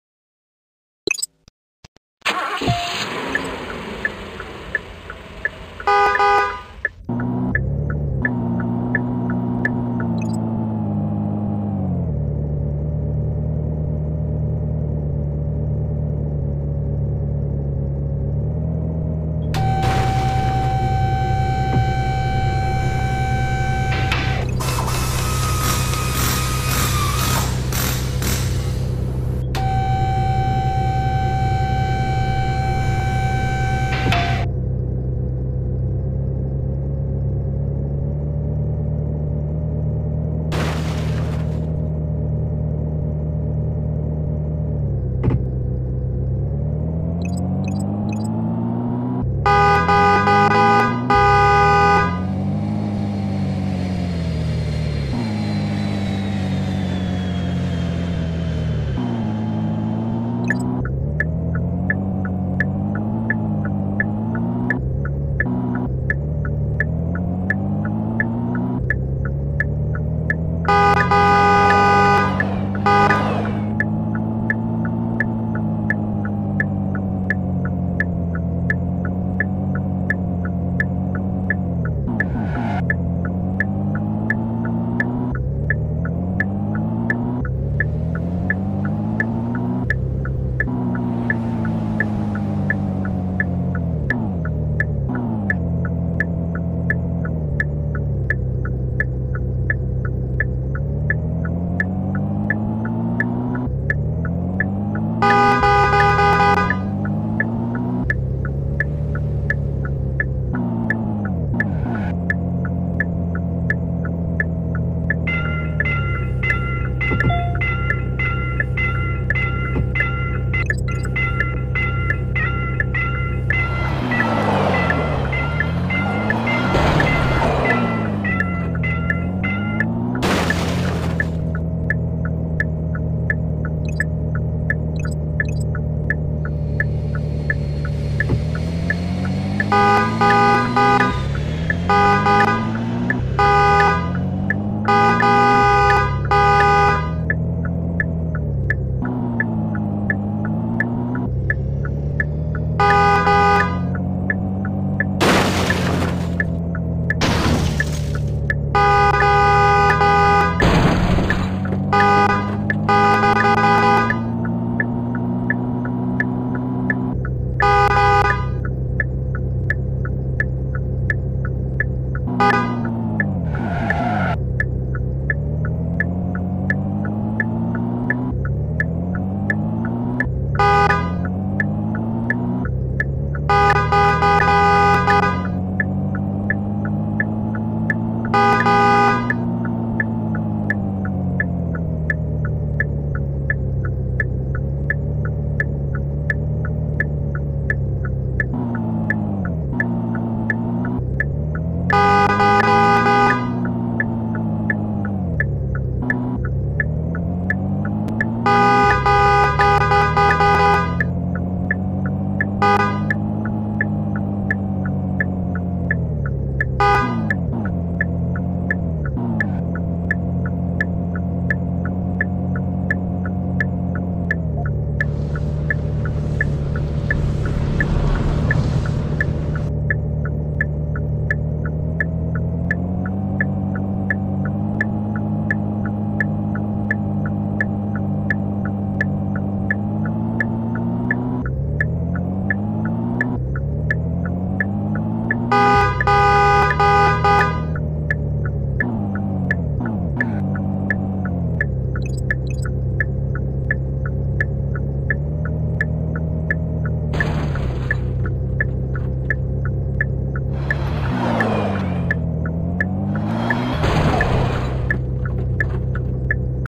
Bullet Train Madness & SUV Auto Wash Station - Funniest Gameplay Ever!
"Experience the ultimate thrill with 'Bullet Train Madness & SUV Auto Wash Station - Funniest Gameplay Ever!' Dive into the chaos and hilarity of high-speed train adventures and car wash mishaps in this side-splitting gameplay video. Perfect for fans of funny gaming moments, fast-paced action, and outrageous scenarios. Don't miss out on the funniest gameplay ever!"